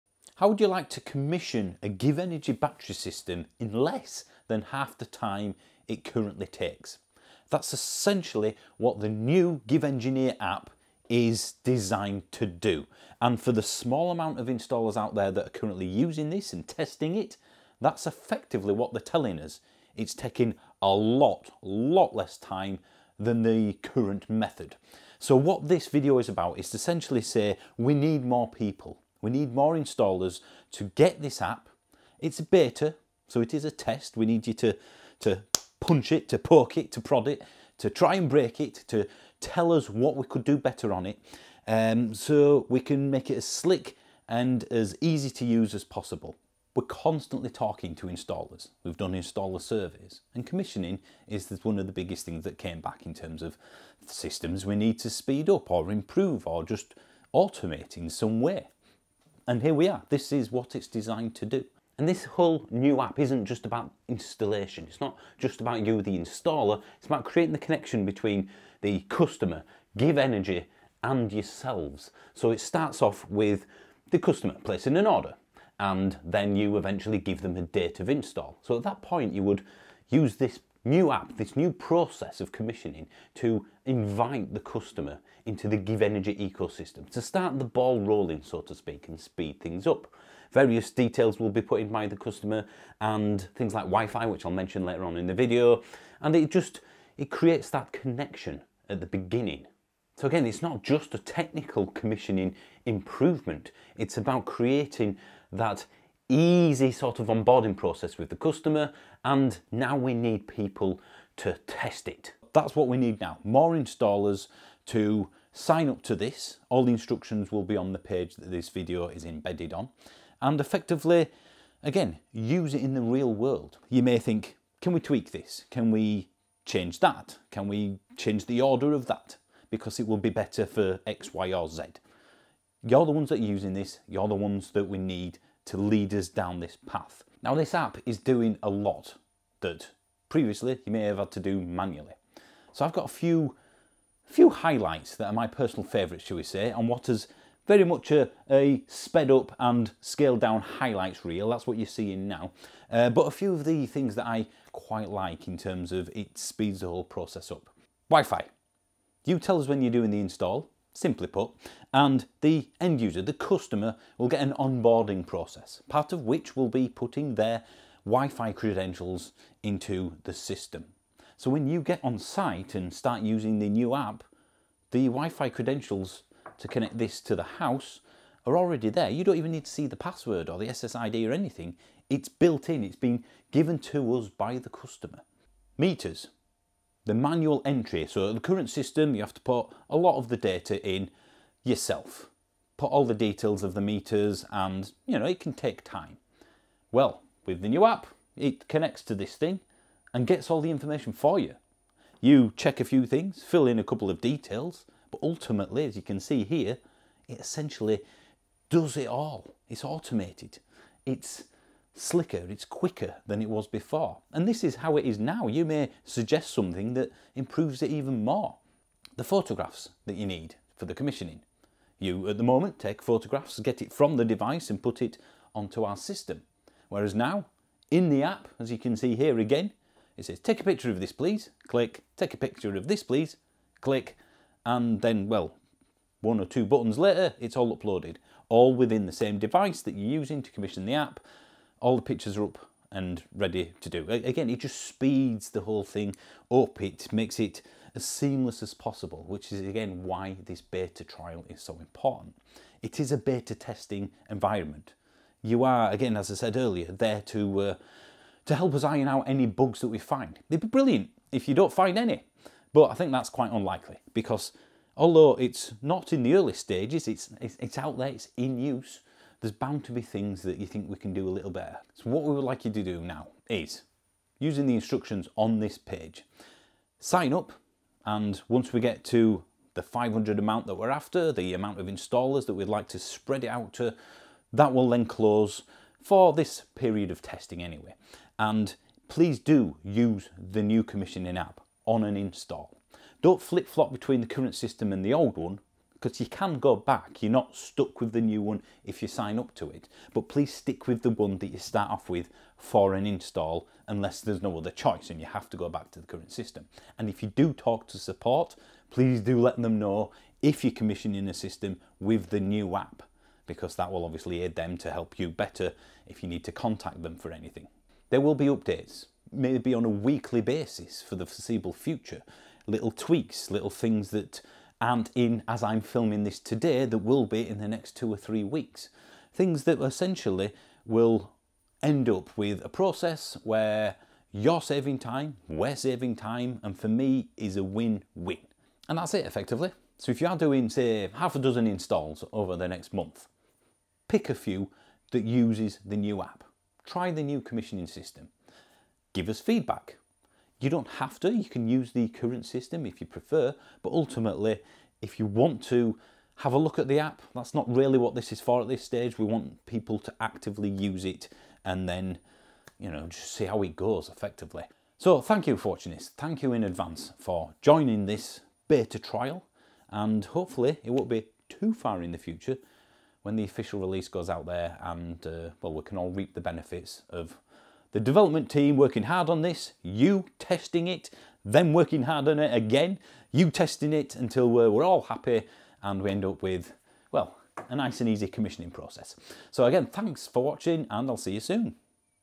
The GivEnergy GivInstaller App is in BETA!
A new way of commissioning is here and we need YOU to help test it! Now with it's new name of the 'GivInstaller App'.